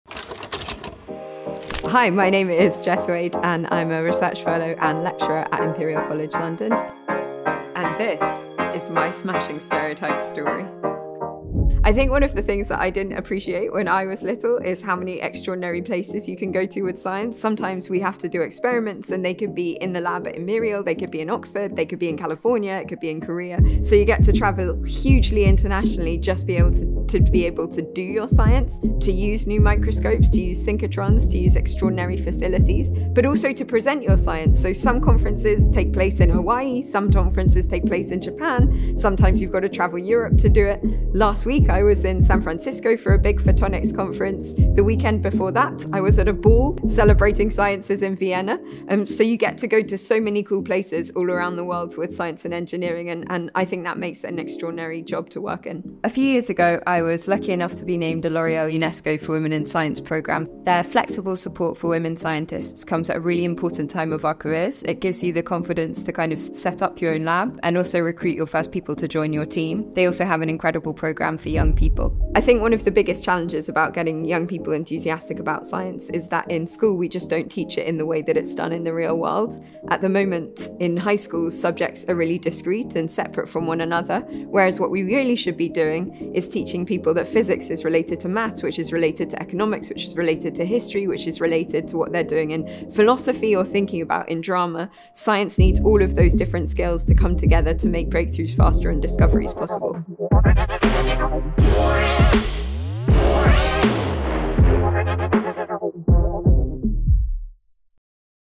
Meet Jess, Scientist | My#SmashingStereotypesstory
Meet Jess Wade, a Research Fellow and Lecturer at Imperial College London, as she shares her Smashing Stereotypes story. Jess is a winner of the L’Oréal-UNESCO For Women In Science Rising Talent award in 2021. From conducting experiments in Imperial's labs to presenting her science at conferences in San Francisco and Vienna, her journey as a scientist has taken her across extraordinary places.

She is also passionate about science education and believes there needs to be change in how it’s taught in schools. According to Jess, more of us need to be aware of the fascinating connection science has with subjects like maths, economics, history, and philosophy.

Each year, the British Science Association runs the Smashing Stereotypes campaign during British Science Week, celebrating the diverse people and careers in science and engineering.

Credits:
Video production: Jack Howard at Crown Videos
Photography: Cebo Luthuli